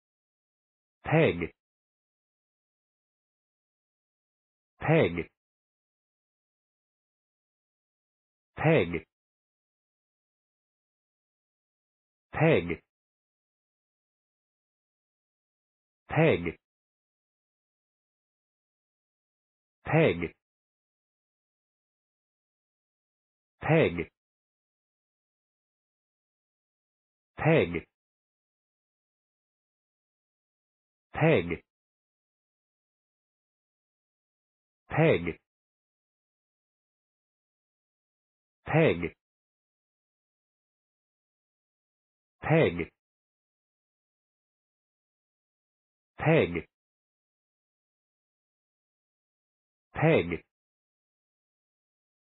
Pronunciation of Peg | Definition of Peg
Peg pronunciation | How to pronounce Peg in English?
/peg/
Meaning of Peg | What is Peg?
(1) (noun) a holder attached to the gunwale of a boat that holds the oar in place and acts as a fulcrum for rowing; (2) (noun) small markers inserted into a surface to mark scores or define locations etc.; (3) (noun) a prosthesis that replaces a missing leg; (4) (noun) a wooden pin pushed or driven into a surface; (5) (verb) succeed in obtaining a position; (6) (noun) informal terms for the leg; (7) (verb) fasten or secure with a wooden pin; (8) (verb) pierce with a wooden pin or knock or thrust a wooden pin into; (9) (noun) regulator that can be turned to regulate the pitch of the strings of a stringed instrument; (10) (verb) stabilize (the price of a commodity or an exchange rate) by legislation or market operations;
What is Peg | Peg meaning and definition?